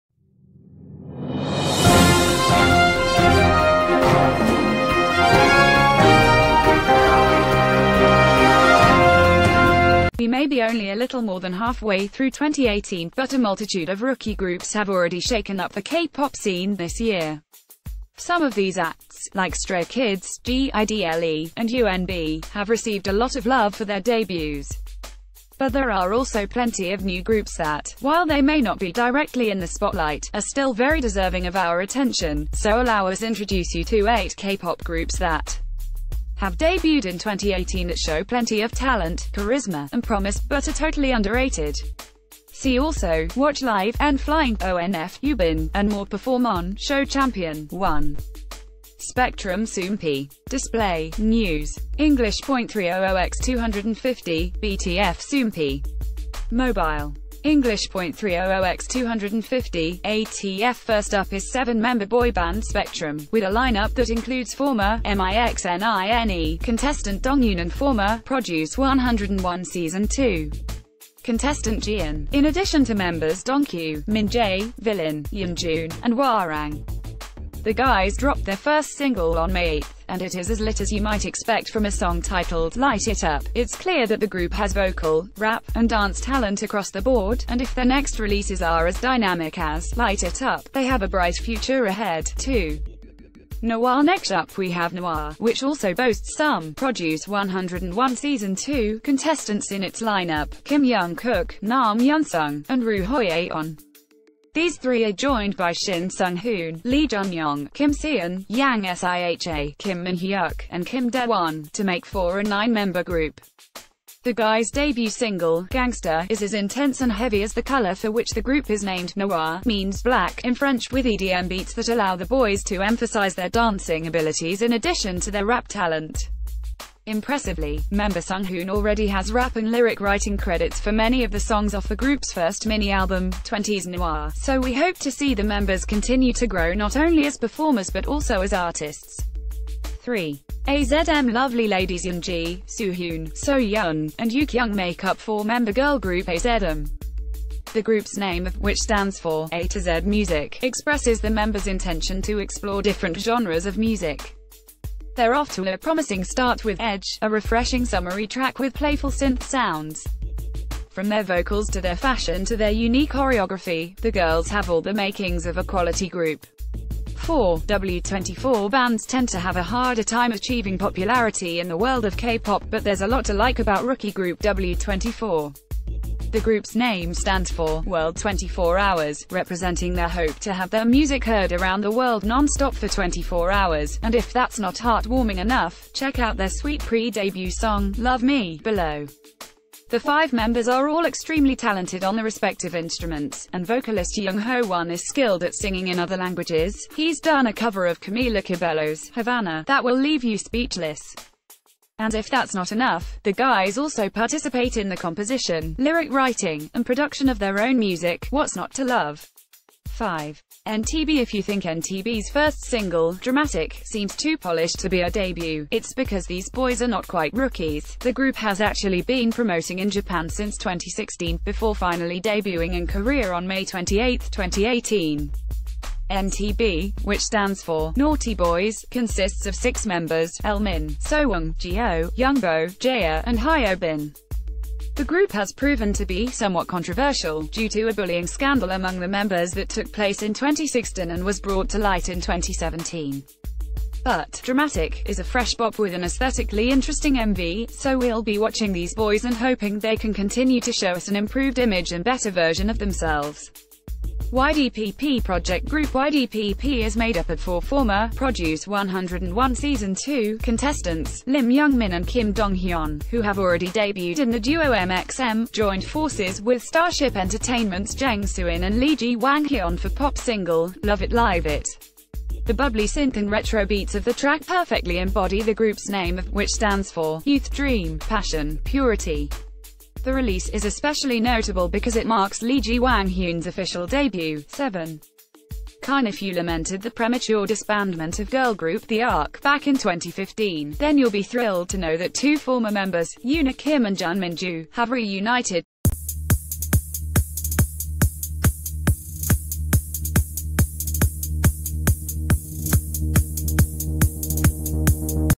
9 Underrated K-Pop Groups That Have Debuted In 2018 So Far
9 Underrated K-Pop Groups That Have Debuted In 2018 So Far
We may be only a little more than halfway through 2018, but a multitude of rookie groups have alread...